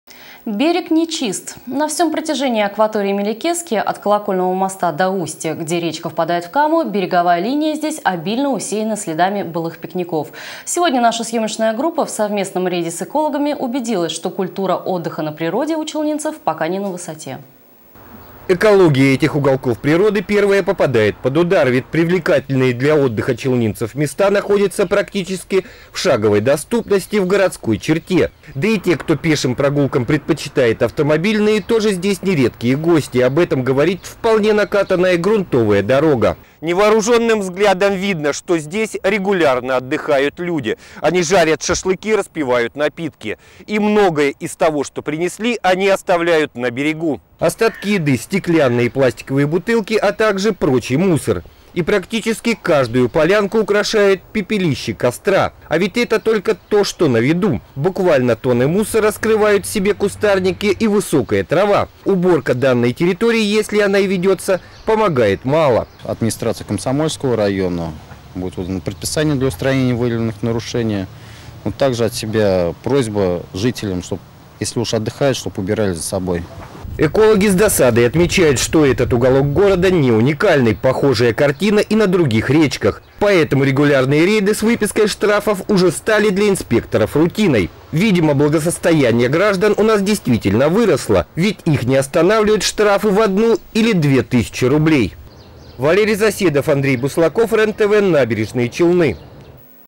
Отдыхать не умеем
На всем протяжении акватории Мелекески -- от Колокольного моста до устья -- береговая линия обильно усеяна следами былых пикников. Сегодня съемочная группа в совместном рейде с экологами убедилась, что культура отдыха на природе у челнинцев пока не на высоте.